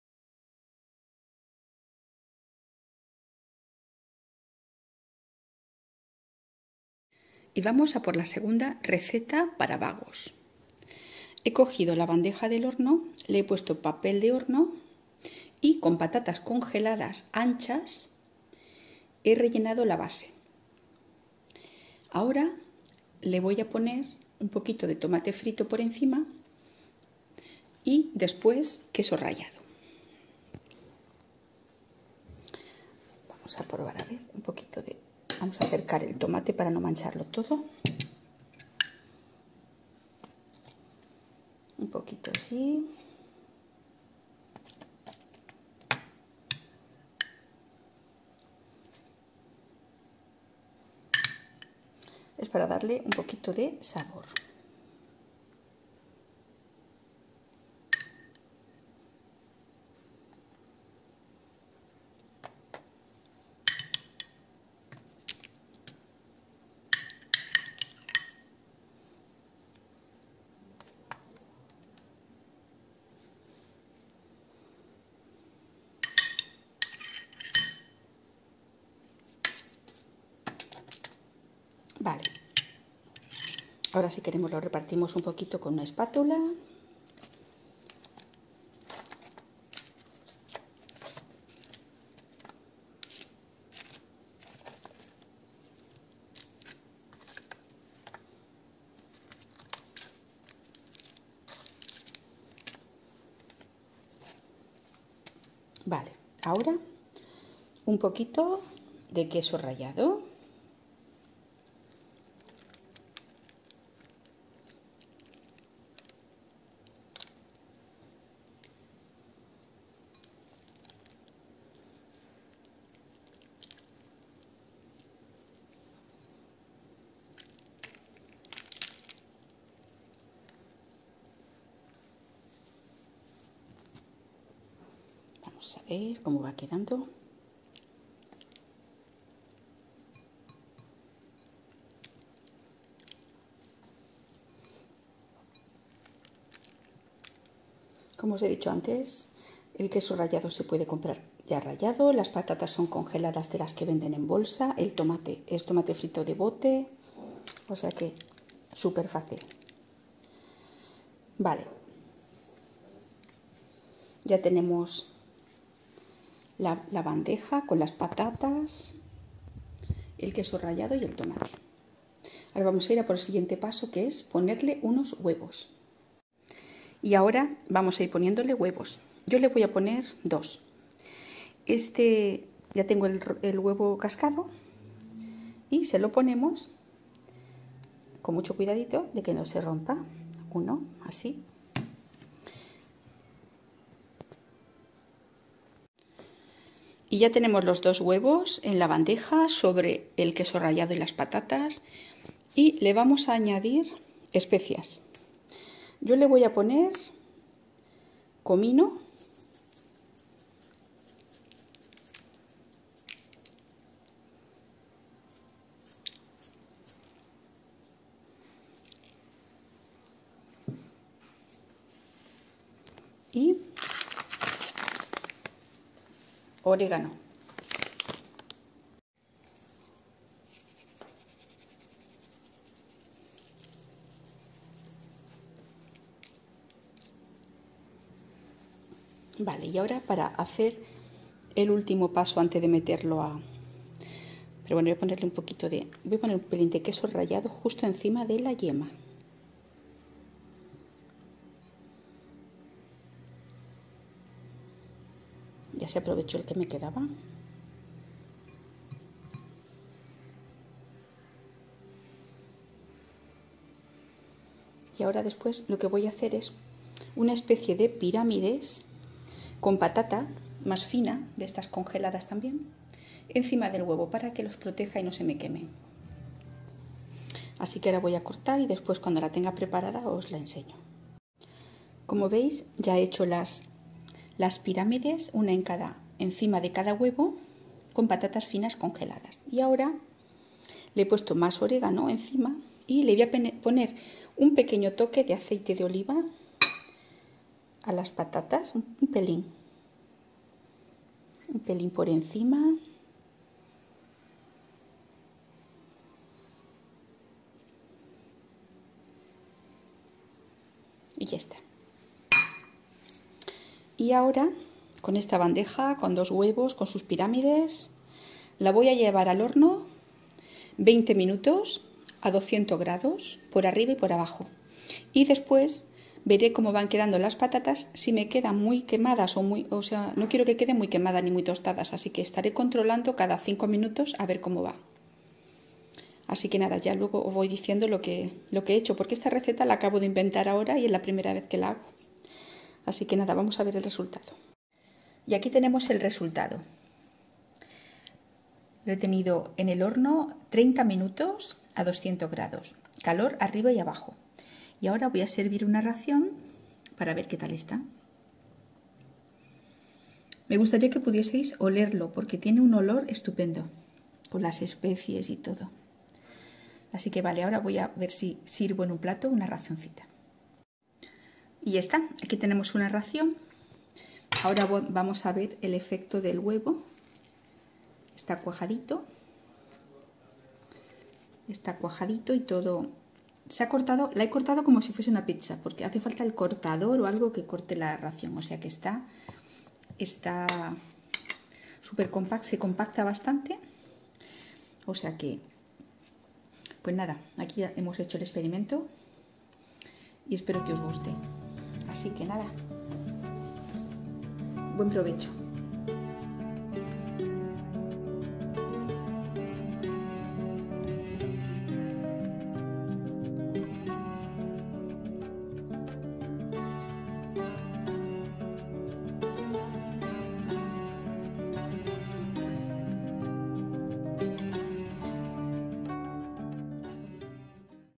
COMO HACER PIZZA de patatas con huevos , Fácil
2ª Receta de "Mariola cocina  para vagos" , pizza de patatas con huevos. Me han pedido que haga recetas para personas que ni saben cocinar ni les apetece hacerlo, con ese nombre Recetas para Vagos. Así empiezo esta sección con esta receta. Espero que os gusten.
1st Recipe of "Mariola cocina para vagos", potato pizza with eggs. I have been asked to make recipes for people who neither know how to cook nor feel like cooking, with that name Recipes for Bums. So I start this section with this recipe. I hope you like them.